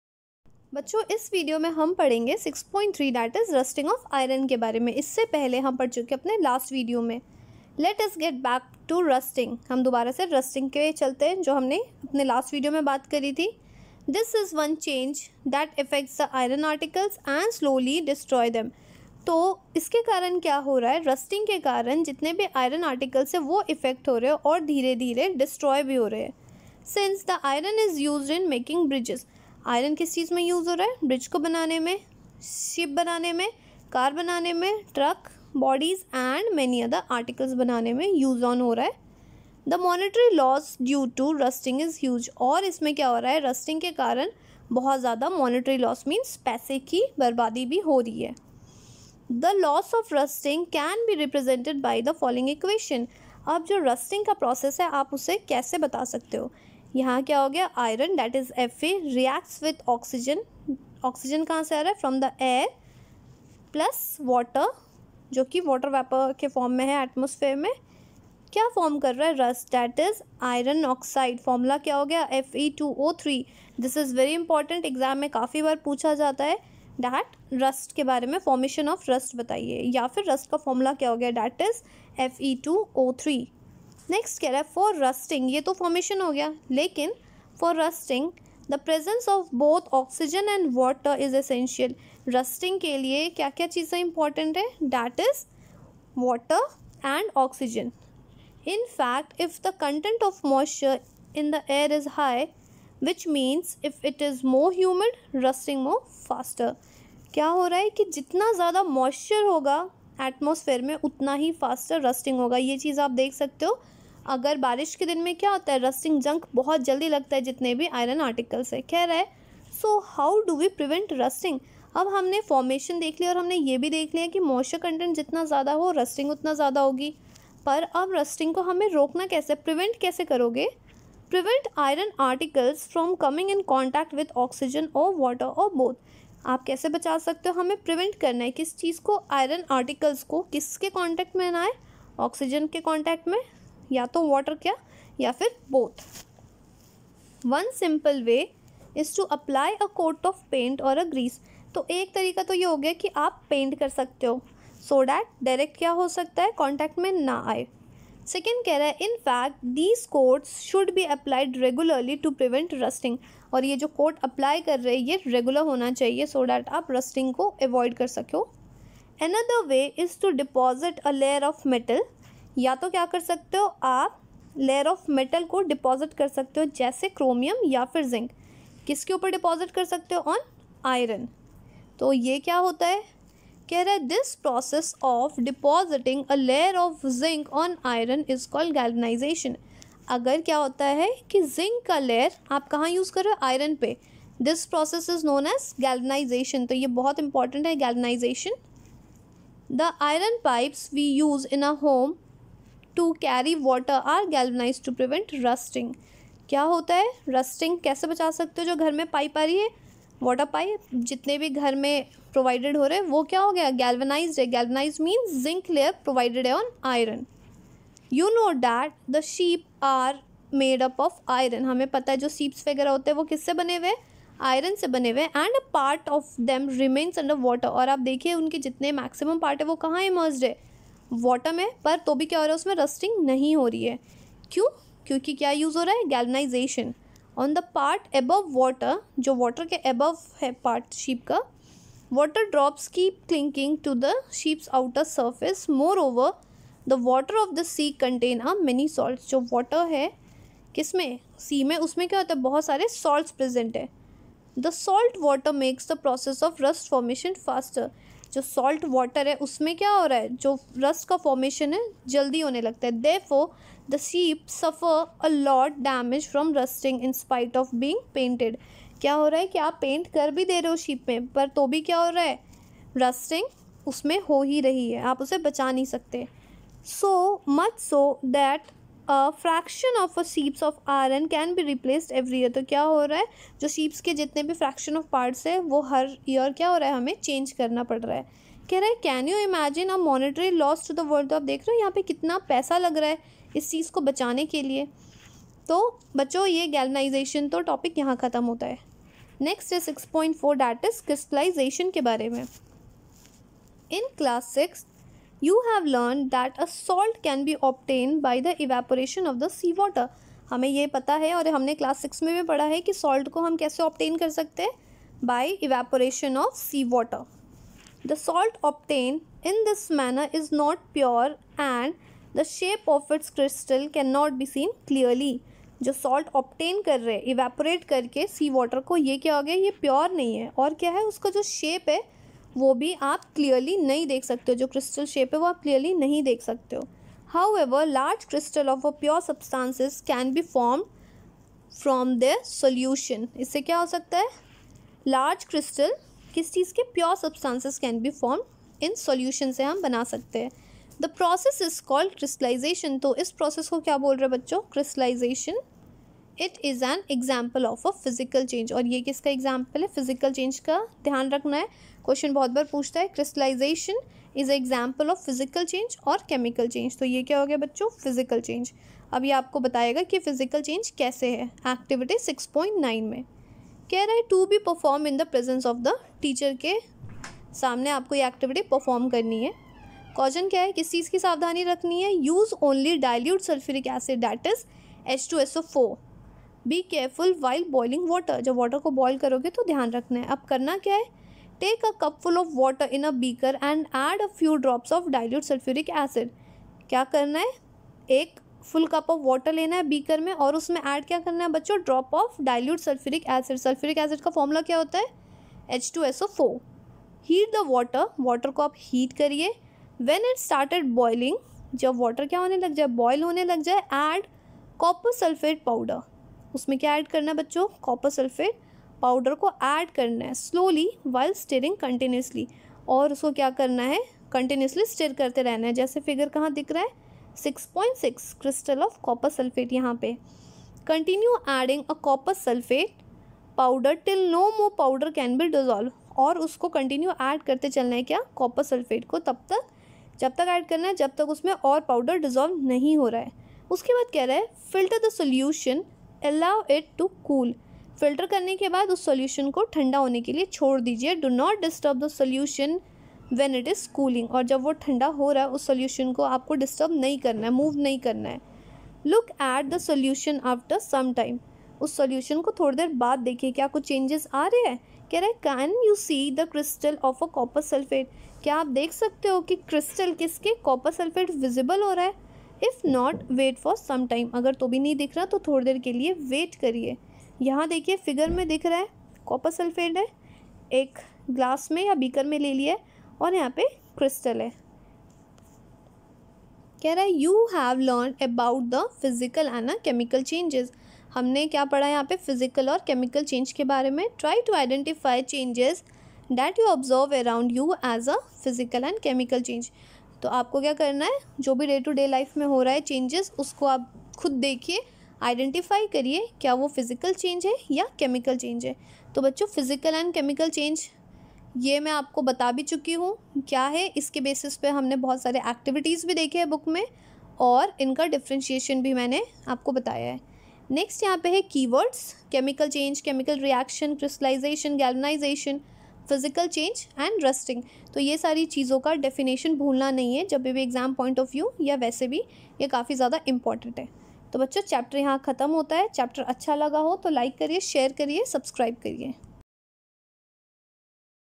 NCERT, Class-7th, Chapter - 6 Physical and Chemical Changes, Part - 4
This video helps you to know about - Rusting of Iron, Galvanisation, Crystallisation.